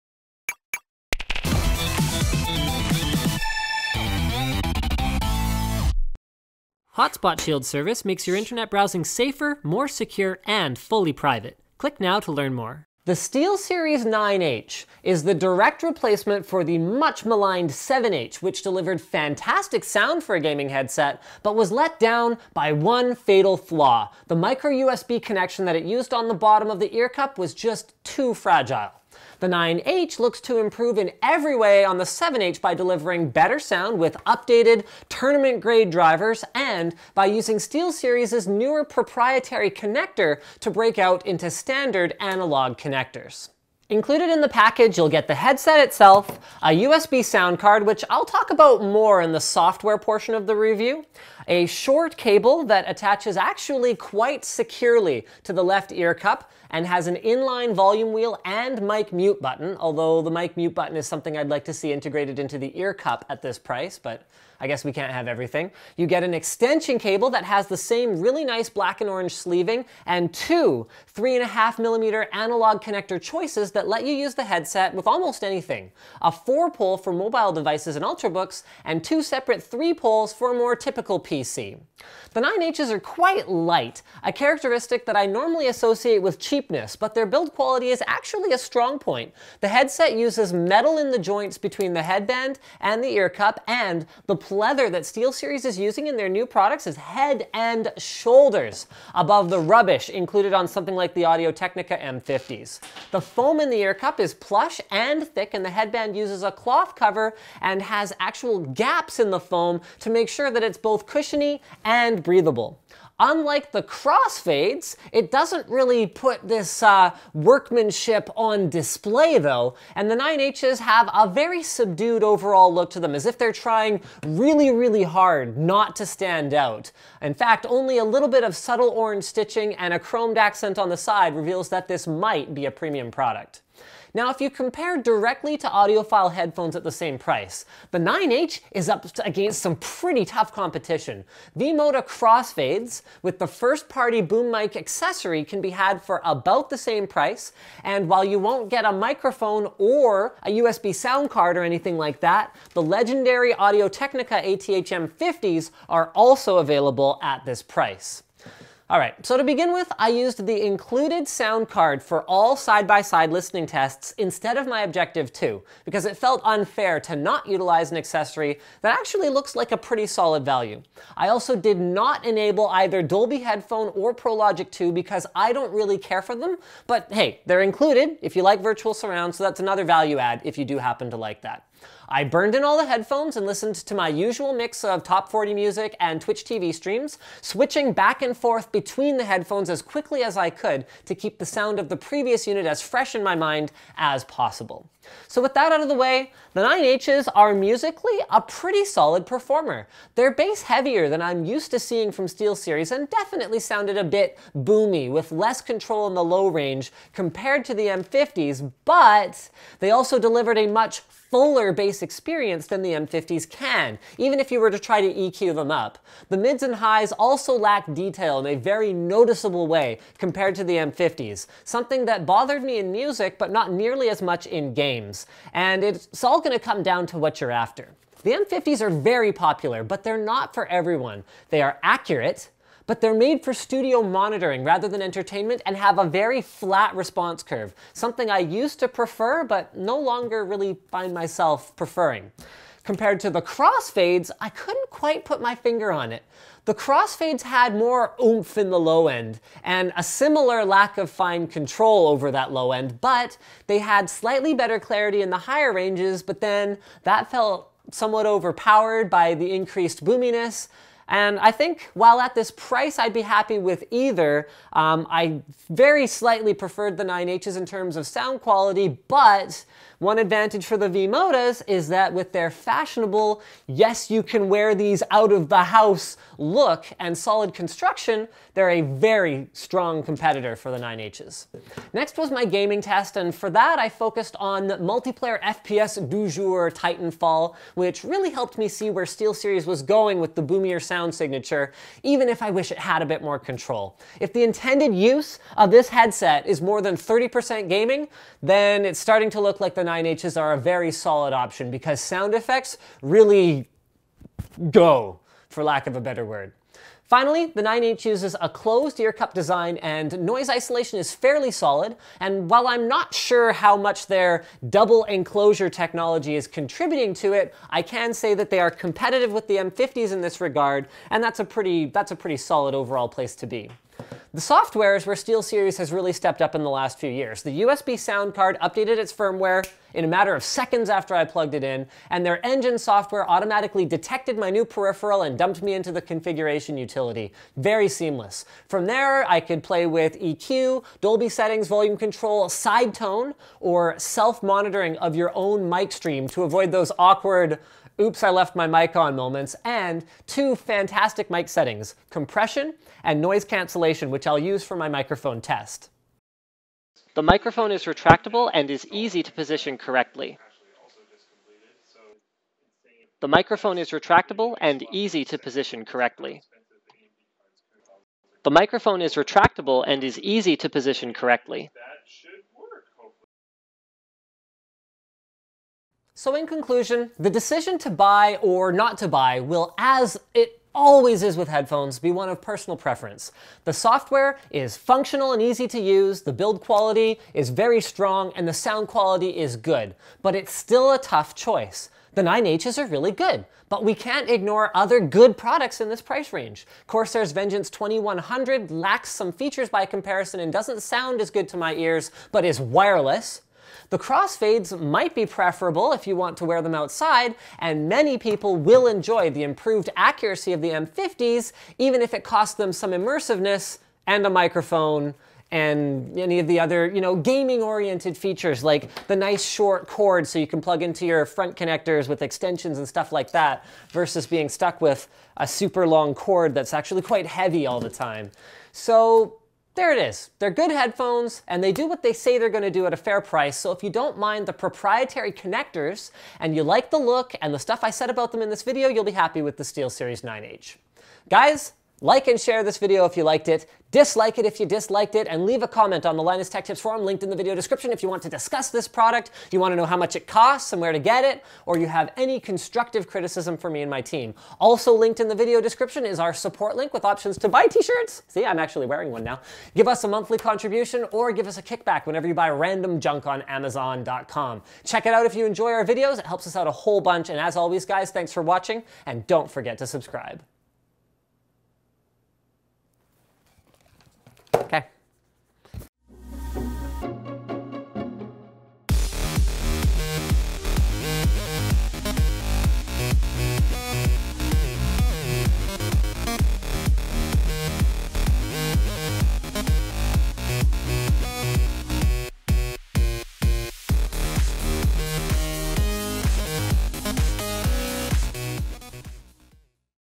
Steelseries 9H Tournament Grade Gaming Headset
My review of the 9H actually ended up more positive than I expected. It's sandwiched in between the 7H, which I really like and the Siberia Elite, which is a much more premium (and also much more expensive) product.